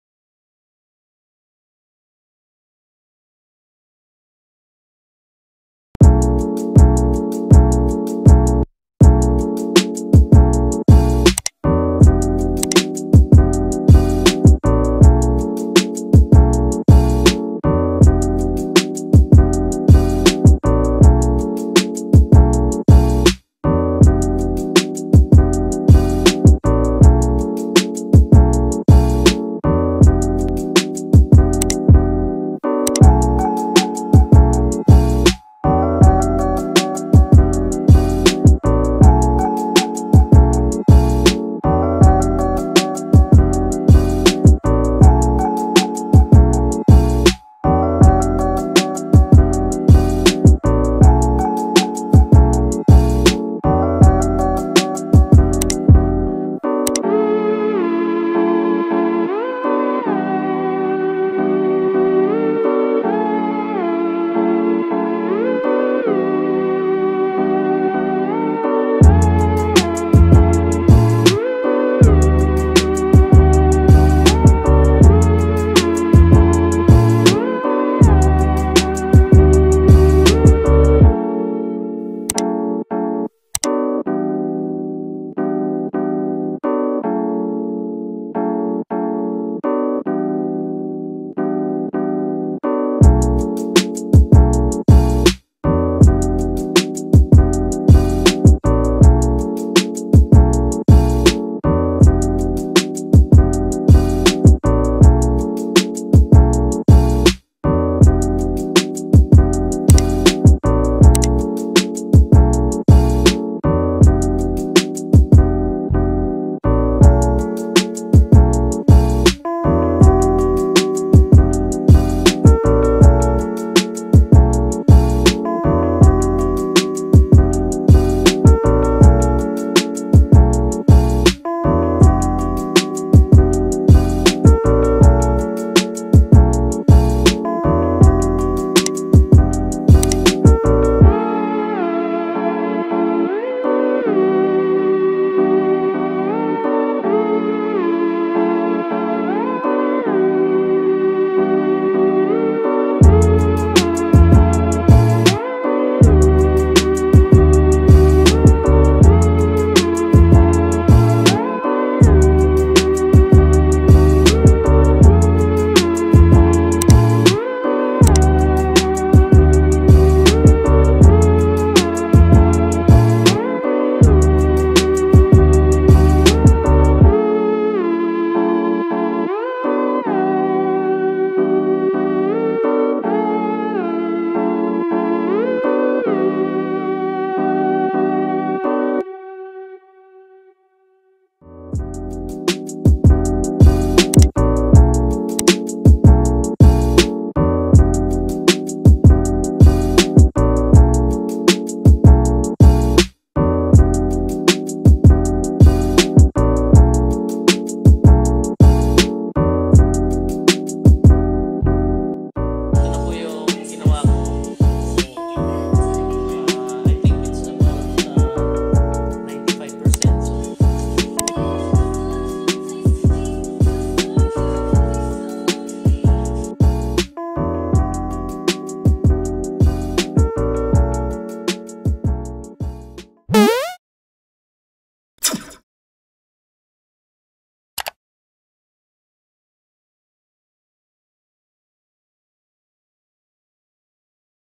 Freehand Realistic Sketching (Timelapse)
In this video I sketched my friend's photo. I hope you find inspiration in this video. It took me 30 mins to finish the sketch. I plan to upload more artworks soon. I hope you enjoy